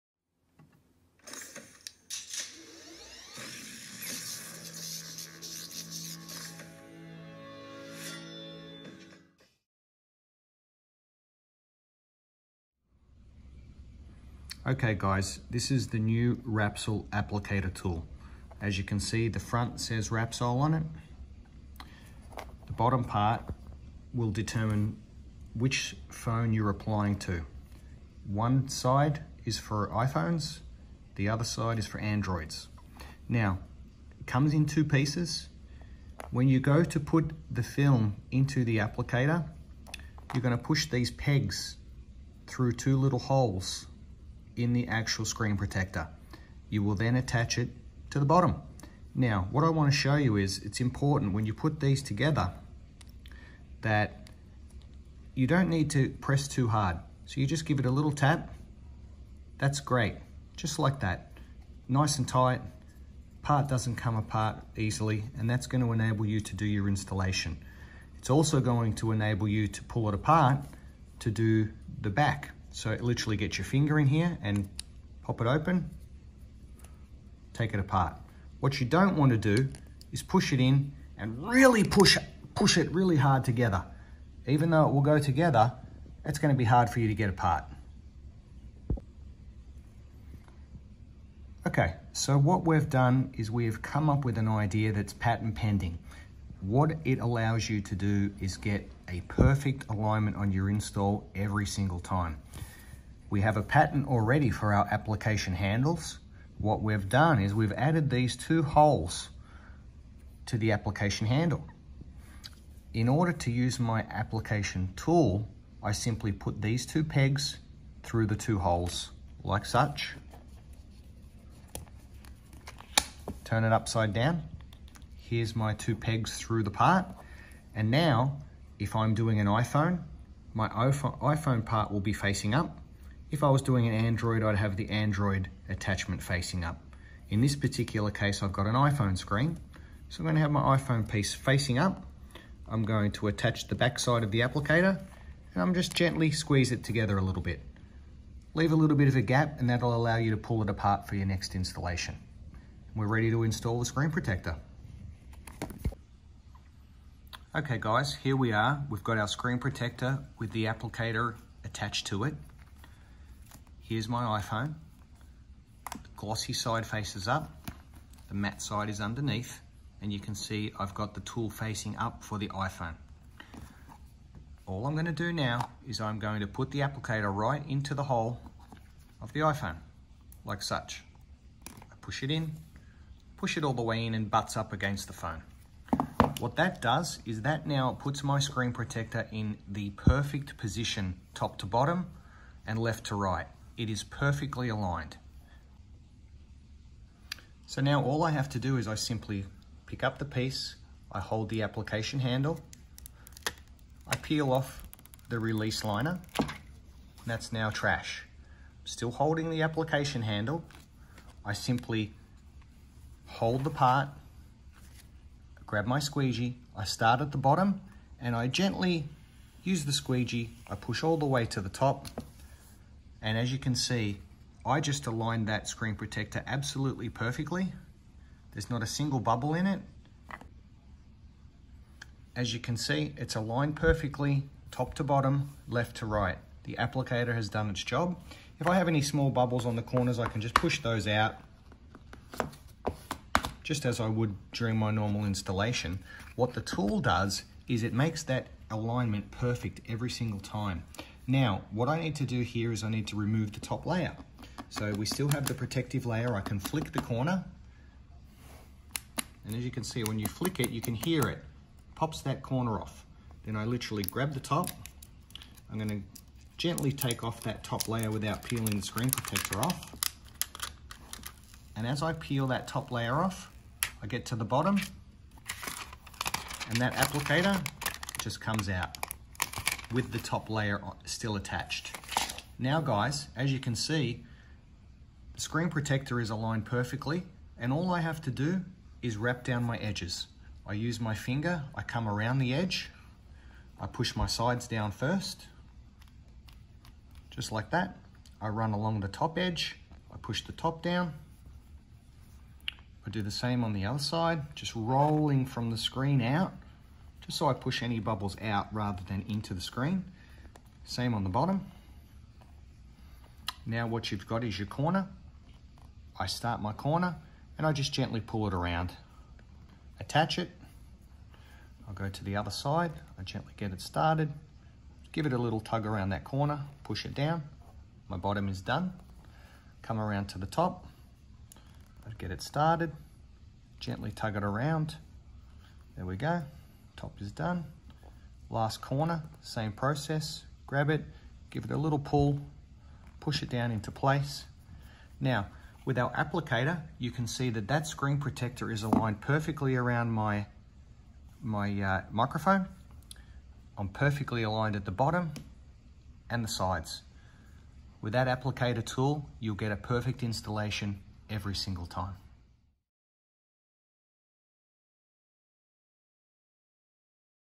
Wrapsol Applicator Tool Installation
Wrapsol's new (patent pending) applicator allows for a perfect install every time!